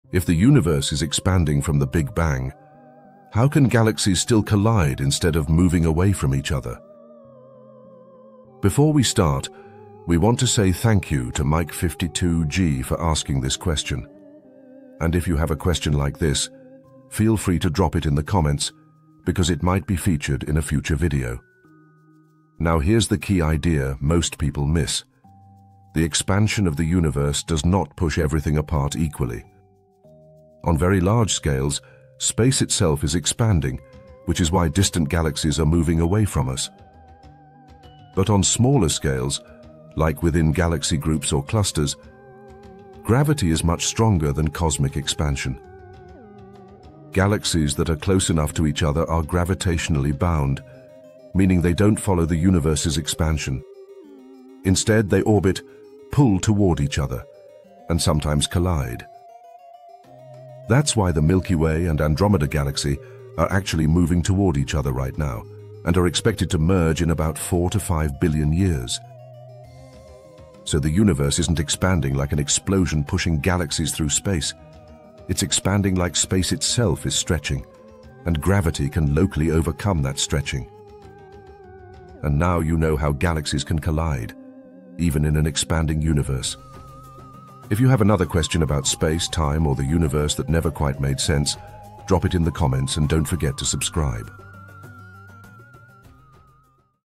If the Universe Is Expanding, How Can Galaxies Collide?
If the universe is expanding, it seems logical to ask how galaxies can still collide instead of moving away from each other.

The answer lies in the difference between cosmic expansion and local gravity.

This short explains how space itself expands on large scales, while gravity keeps nearby galaxies bound together — allowing collisions like the future merger between the Milky Way and Andromeda.

Thanks to @Mike52G  for the great question.

💬 Have a question about space or the universe? Drop it in the comments — it might be featured in a future video.